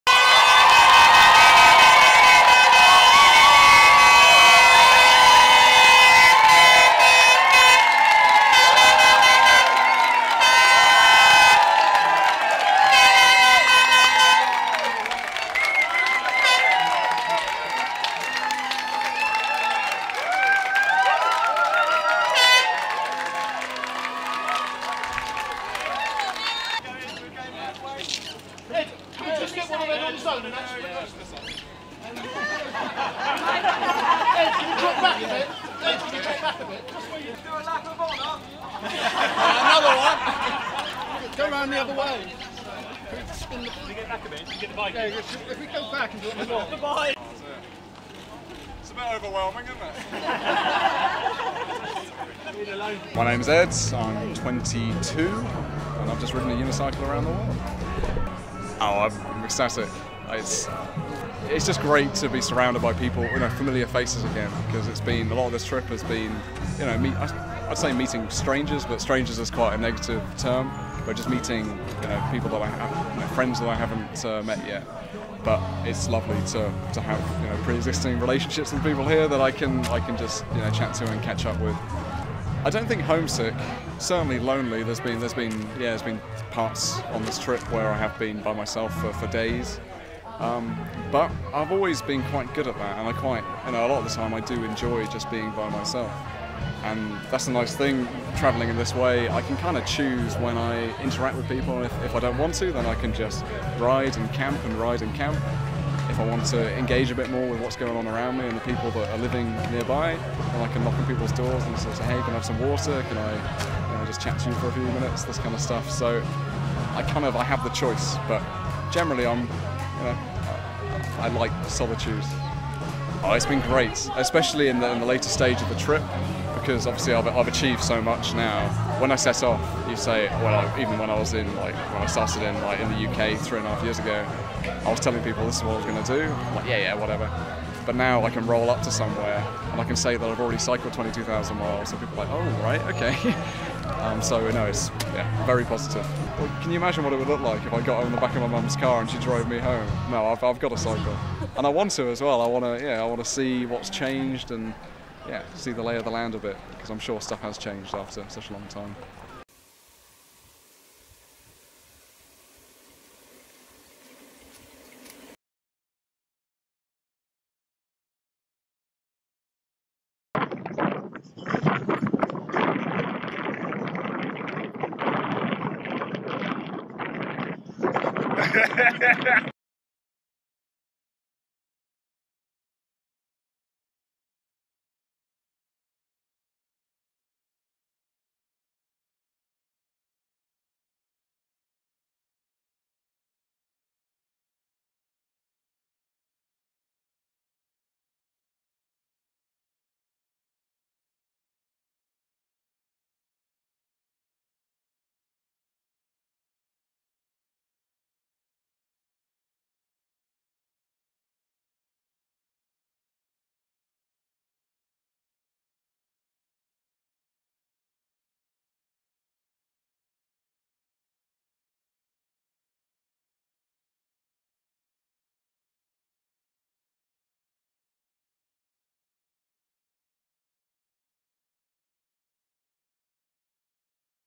Unicyclist completes round the world challenge
The first man to unicycle round the world said he will cycle another 10 miles home after his welcome party as he made his triumphant one-wheeled homecoming.